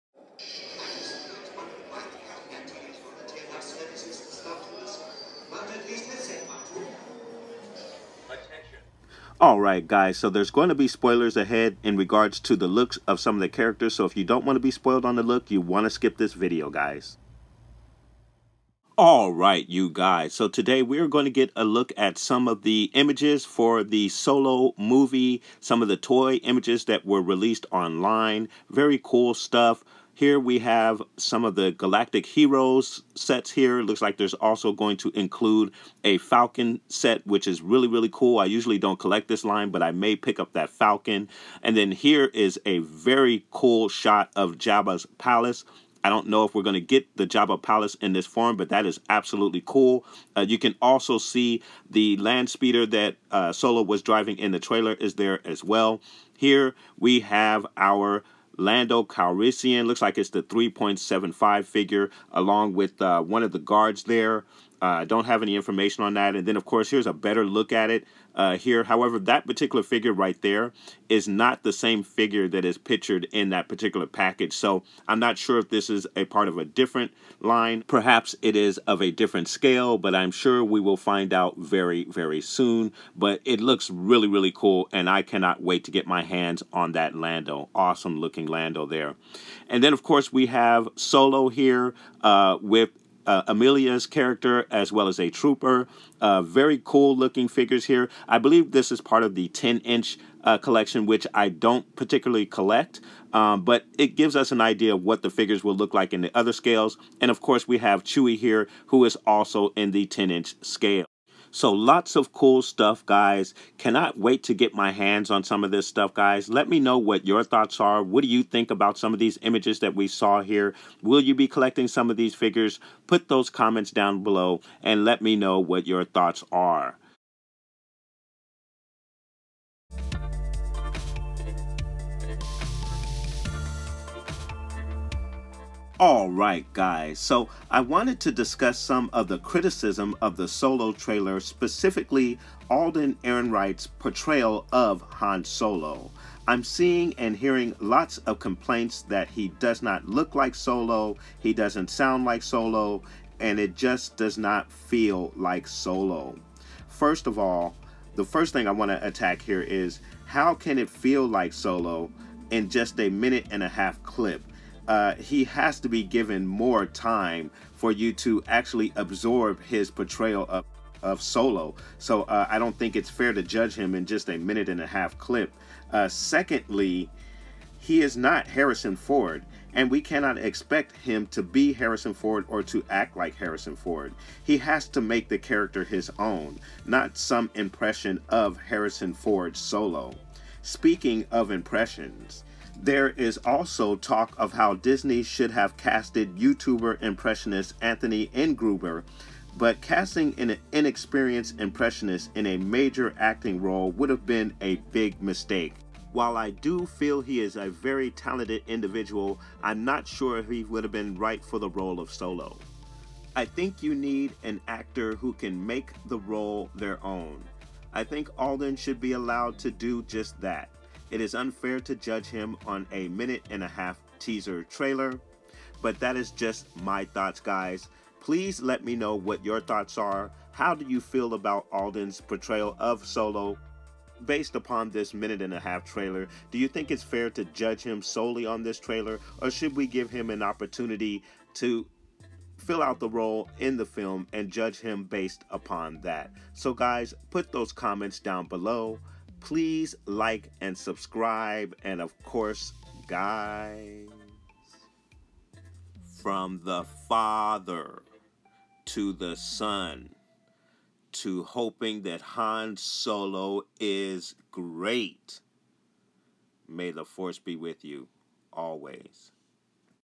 STAR WARS TOY NEWS SOLO FIGURE IMAGES
Today we take a look at leaked photos of some upcoming releases from the Solo film. I also will give some thought on the Solo trailer and the reactions.